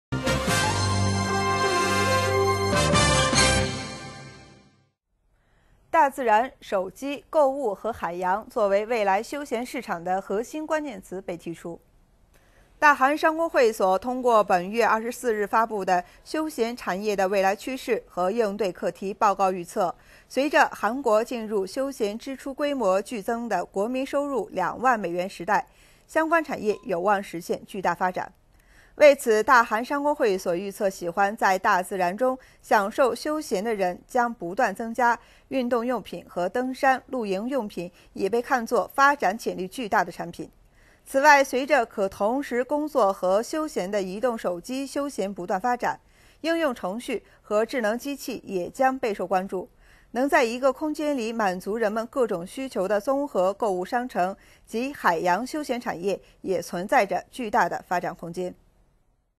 提出休闲产业的未来趋势
大自然(Mother nature)、手机(Mobile)、购物(Malling)和海洋(Marine)作为未来休闲市场的核心关键词被提出。

大韩商工会议所通过本月24日发布的《休闲产业的未来趋势和应对课题》报告预测，随着韩国进入休闲支出规模剧增的国民收入2万美元时代，相关产业有望实现巨大发展。

对此，大韩商工会议所预测，喜欢在大自然中享受休闲的人将不断增加，运动用品和登山、露营用品也被看作为发展潜力巨大的产品。

此外，随着可同时工作和休闲的移动手机休闲不断发展，应用程序和智能机器也将备受关注，能在一个空间里满足人们各种需求的综合购物商城及海洋休闲产业也存在着巨大的发展空间。